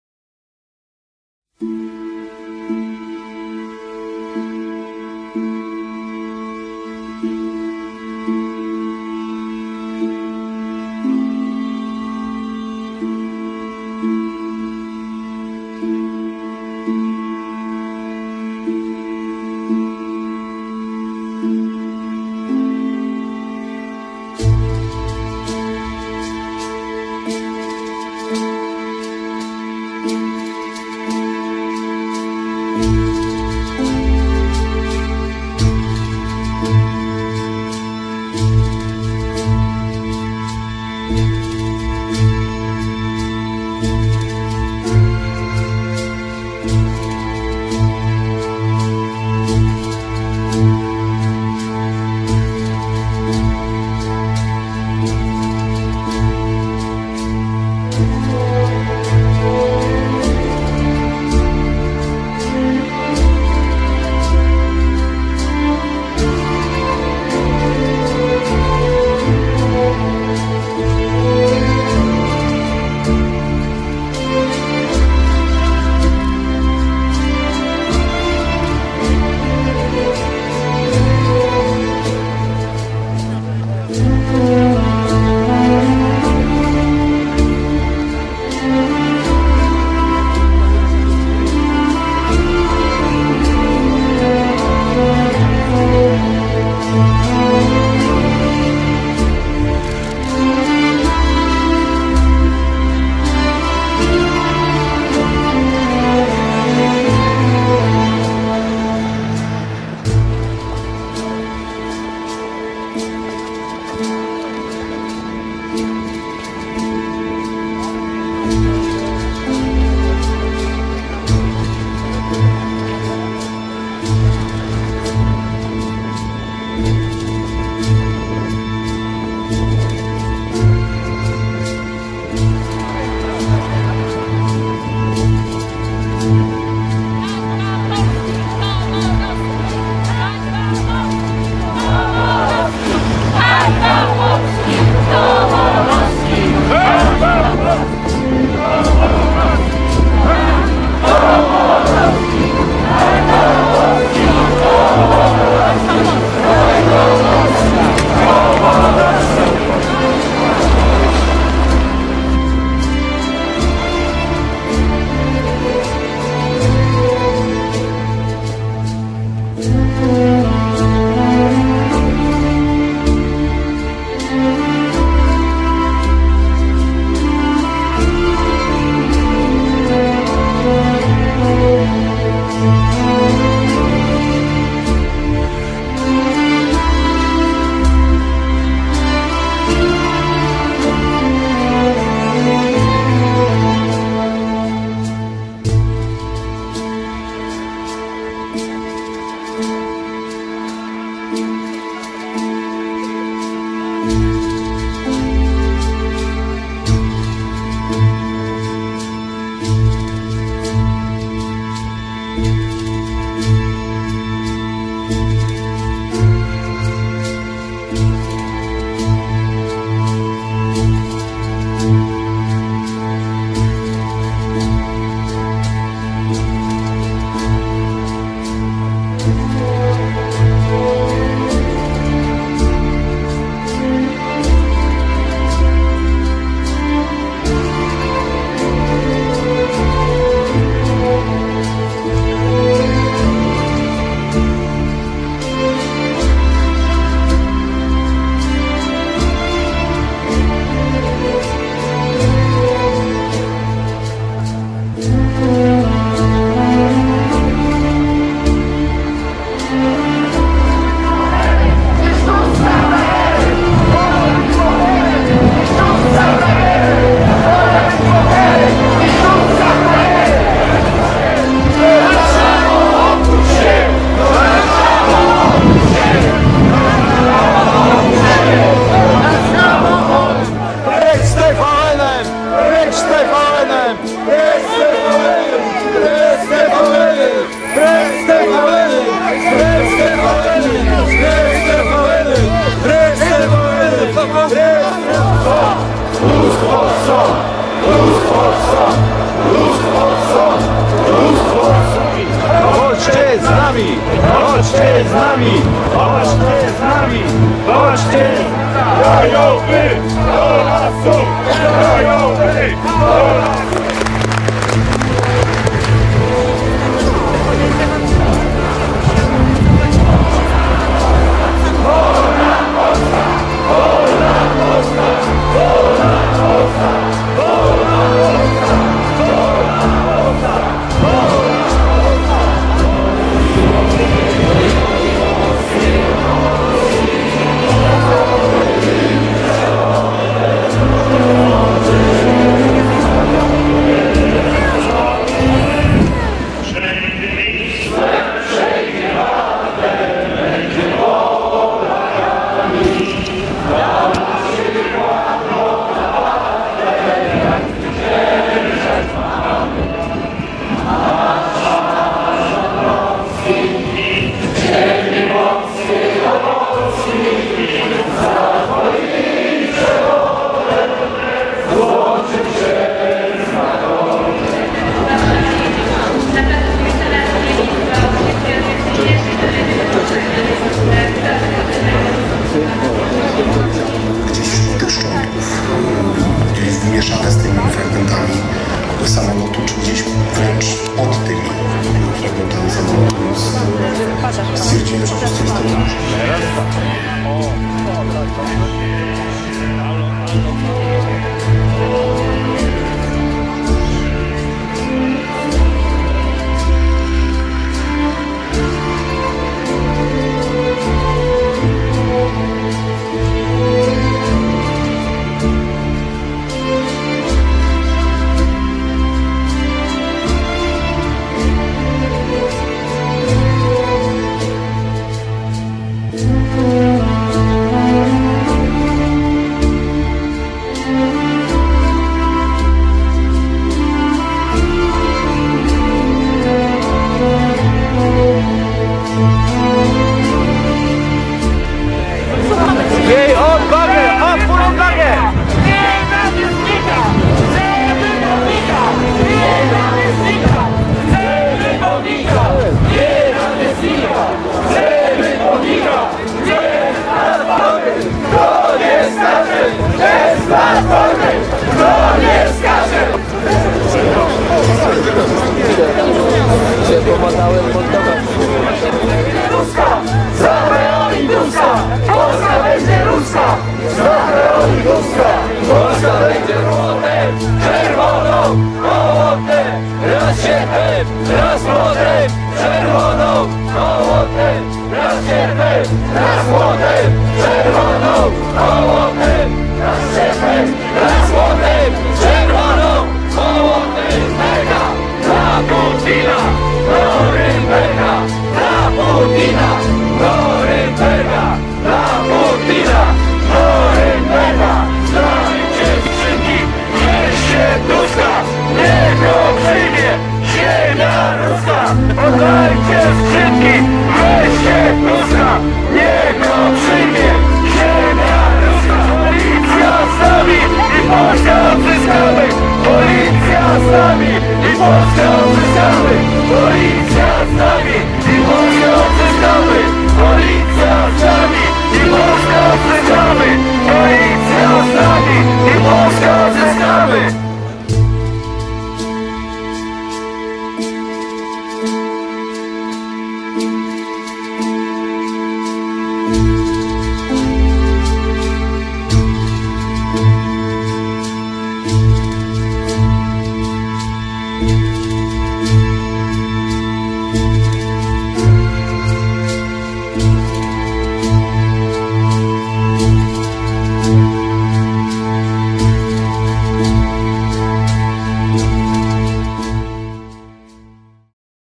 Solidarni 2010 - Krakowskie Przedmieście - Rocznica Tragedii Narodowej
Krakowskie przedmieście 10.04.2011
Media głównego nurtu okłamują nas wszystkich. Tym filmem chcę pokazać prawdę o ludziach. Prawdziwych Polakach. O ludziach którym zależy na przyszłości naszego kraju. Na przyszłości naszych dzieci i wnuków.
Nie dajmy się manipulować mediom.
Wyłączcie telewizory - Włączcie myślenie.